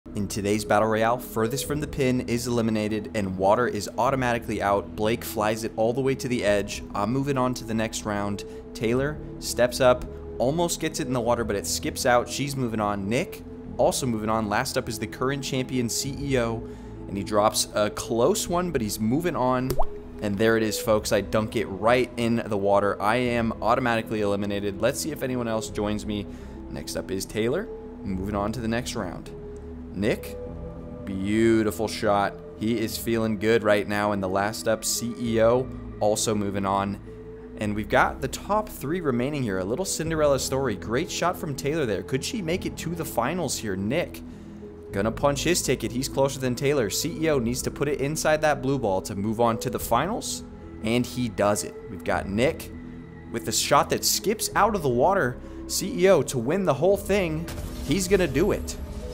Intense Chip N Stick Golf BATTLE ROYALE
Today we have a golf knockout challenge with the Chip N Stick. Who will get the Victory Royale?